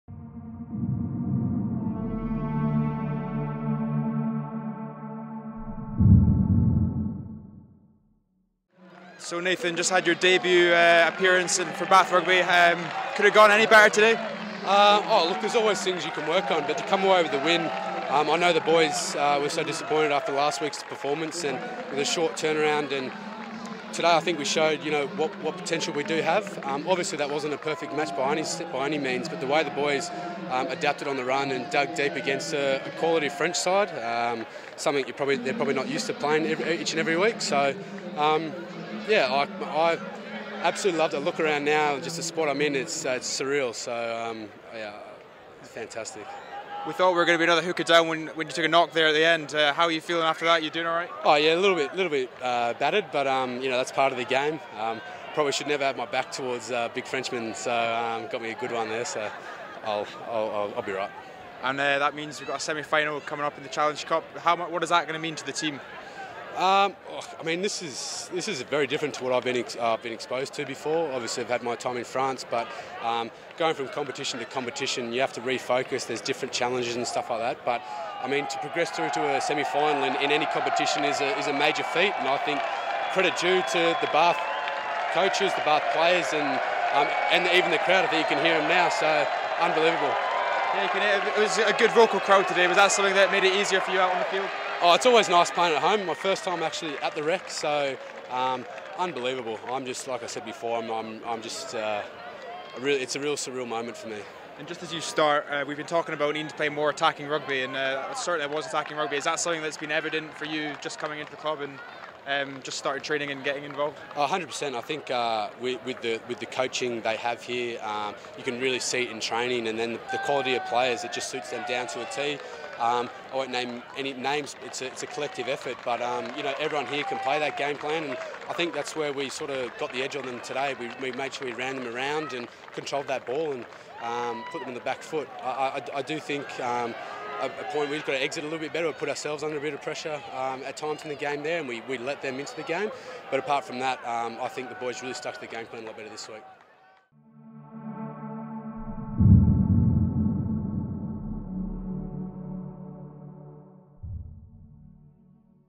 BRTV | Match Review - Bath Rugby V Brive 1st April
Hooker Nathan Charles spoke to BRTV after making his debut for Bath Rugby as the Club secured its place in the European Rugby Challenge Cup Semi-Finals, courtesy of 32-20 victory against Brive.